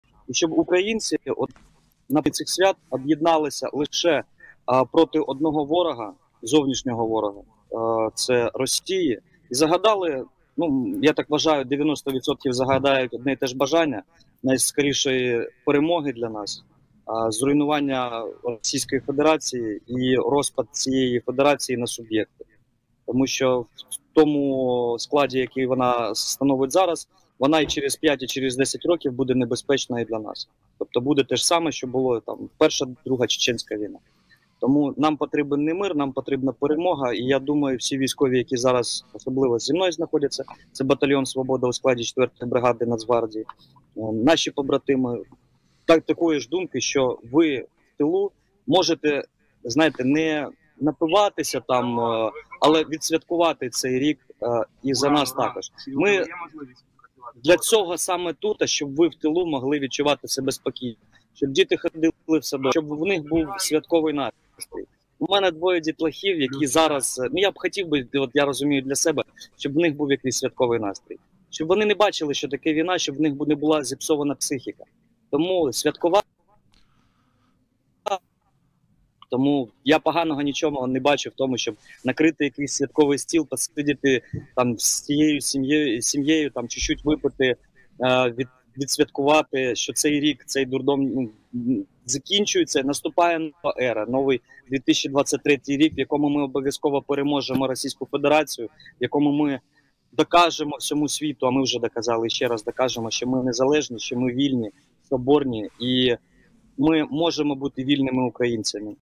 Загадайте бажання про перемогу України і зруйнування російської федерації, — Євгеній Оропай
Я і мої хлопці вважаємо, що ви маєте зібратися за святковим столом, загадати бажання про найскорішу Перемогу, зруйнування російської федерації. Відсвяткуйте і за нас — бо ми тут саме для того, аби ви в тилу відчували себе спокійно, — Євгеній Оропай, лейтенант «Легіону Свободи», командир роти 3-го батальйону оперативного призначення «Свобода» у складі 4-ї бригади оперативного призначення Національної гвардії України зі Східної операційної зони. Етер телеканалу «Еспресо» 31 грудня 2022 року.